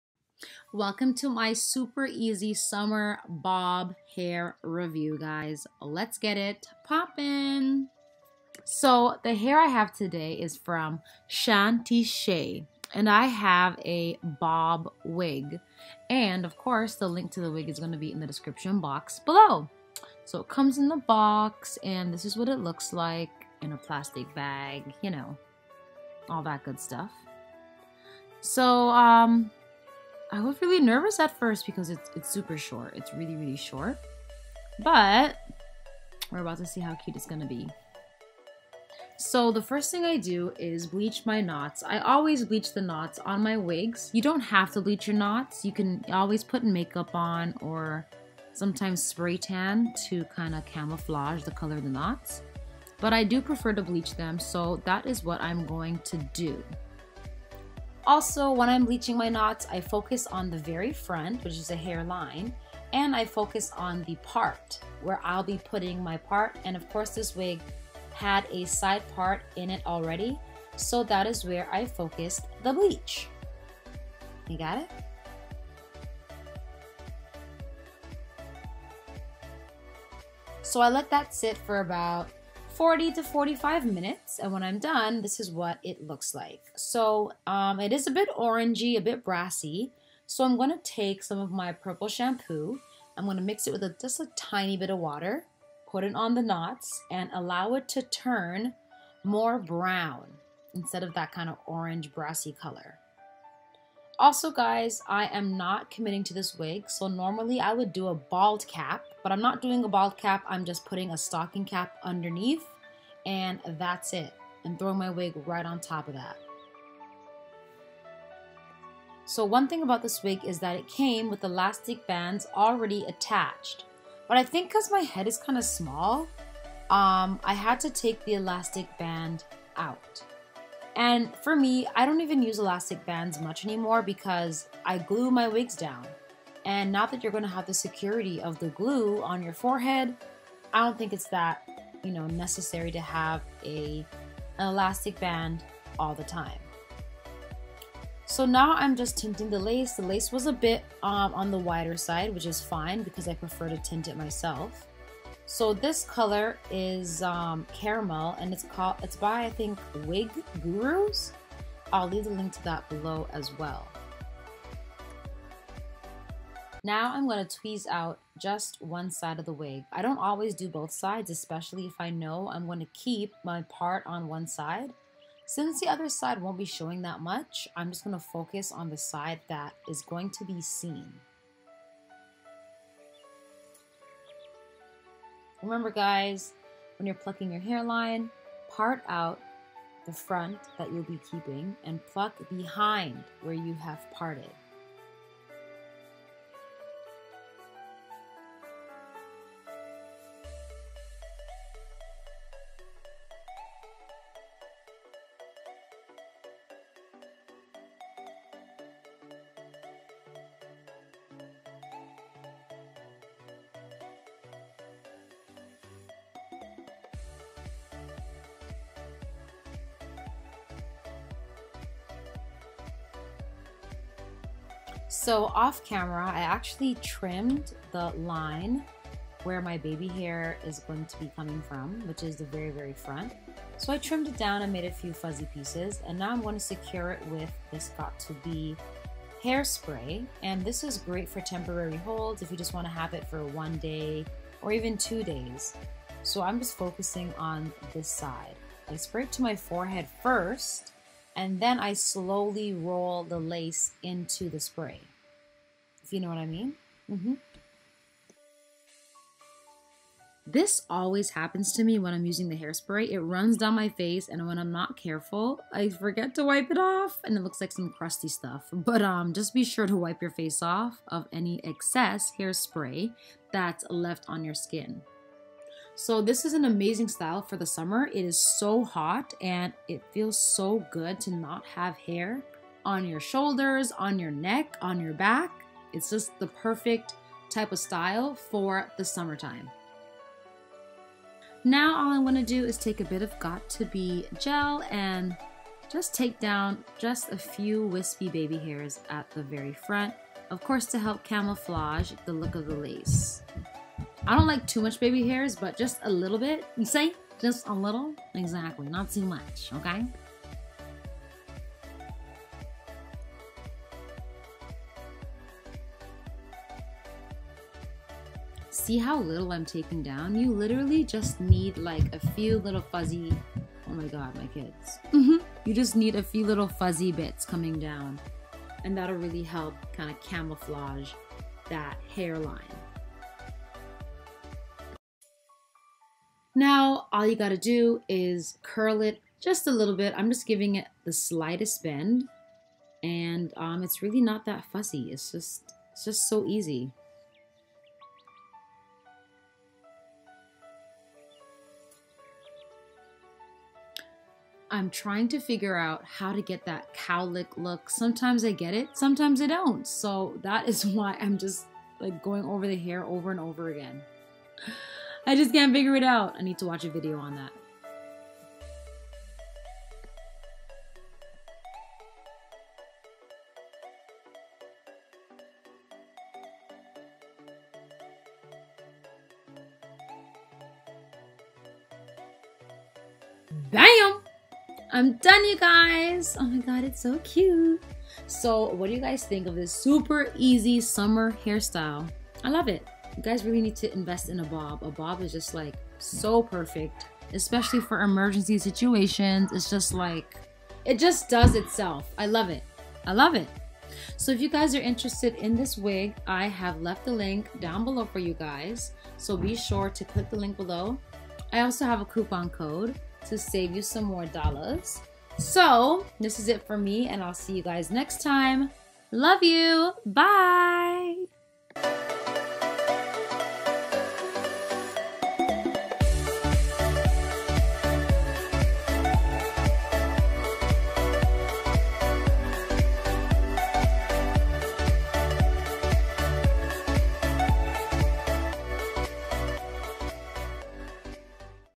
ROCHELLE Recommend Natural Color Short Bob Wig For Summer 8inch 150% density Lace Front Wig
100% Human Hair Wig
Style: Lace Front Wig
Length: 8inch
Color: Natural Color
Density: 150%
Texture: Straight
Cap Size: Medium
Lace Color: Medium Brown